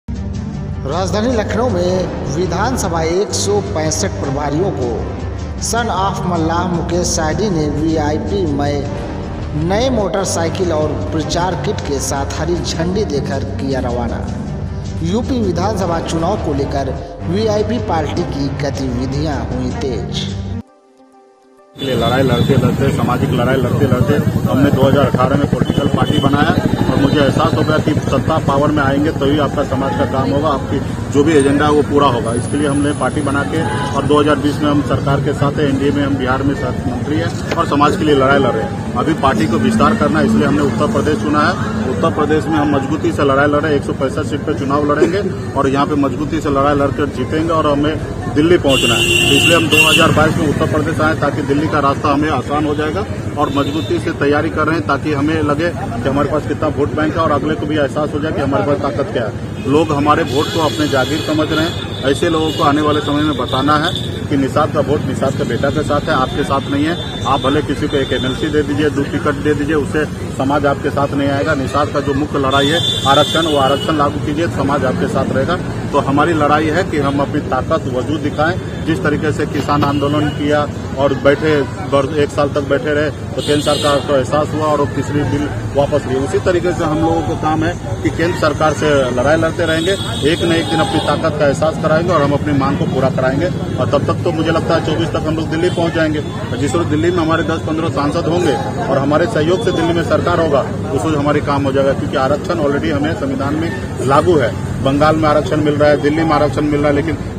विधानसभा 165 प्रभारी को सन ऑफ मल्लाह मुकेश सहनी ने वीआईपी मय नये मोटर साईकल,
विधानसभा 165 प्रभारी को सन ऑफ मल्लाह मुकेश सहनी ने वीआईपी मय नये मोटर साईकल और प्रचार किट के साथ हरी झंडी देकर किया रवाना,